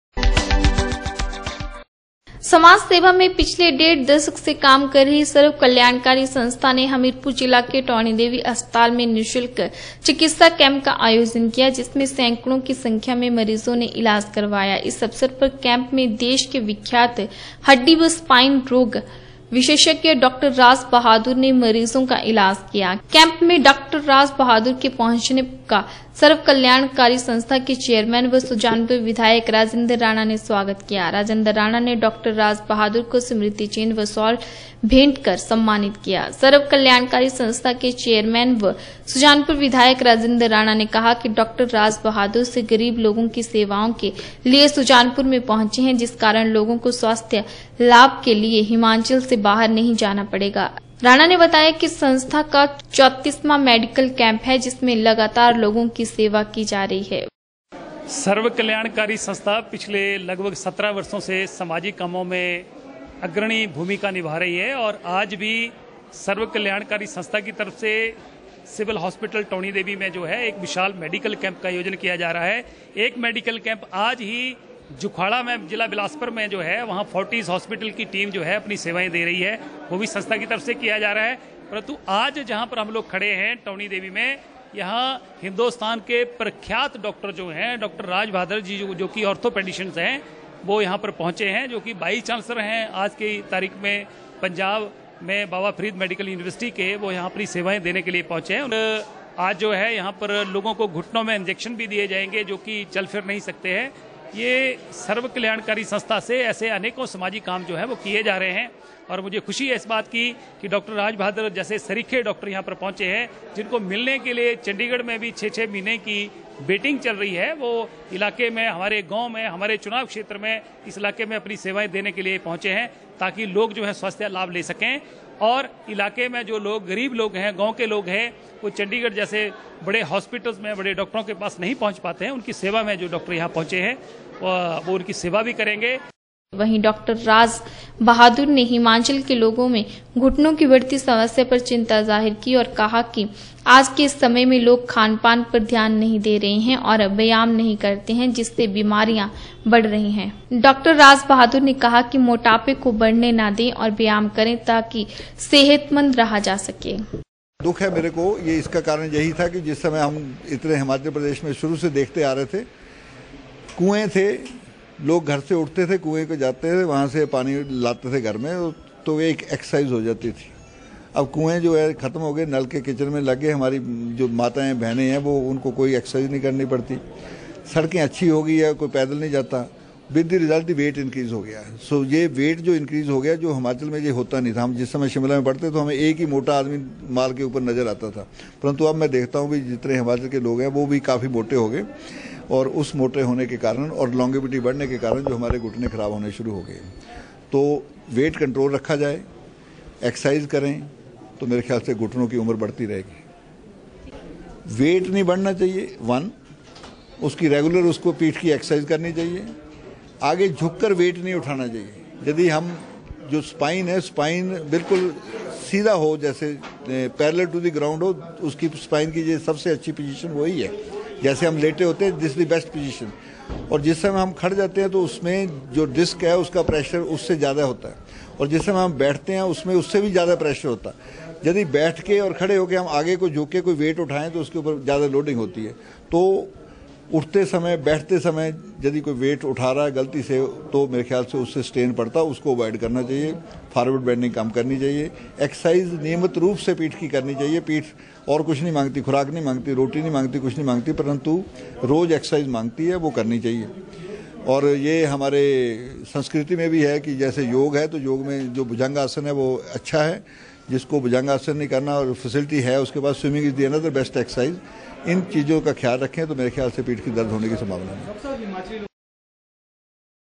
सर्वकल्याणकारी संस्था ने हमीरपुर जिला के टौणी देवी अस्पताल में निशुल्क चिकित्सा कैंप
समाज सेवा में पिछलेडेढ दशक से काम कर रही सर्वकल्याणकारी संस्था ने हमीरपुर जिला के टौणी देवी अस्पताल में निशुल्क चिकित्सा कैंप का आयोजन किया जिसमें सैकडों कीसंख्या में मरीजोंने इलाज करवाया। इस अवसर पर कैंप मेंदेश केविख्यात हडडी व स्पाइन रोग विशेषज्ञ डा राज बहादुर ने मरीजों का इलाज किया। कैंप में डा राज बहादुर के पहुंचने का सर्वकलयाणकारी संस्था के चेयरमैन एवं सुजानपुर विधायक राजेन्द्र राणा ने स्वागत किया। राजेन्द्र राणा ने डा राज बहादुर को स्मृति चिन्हव शाल भेंट कर भी सम्मानित किया।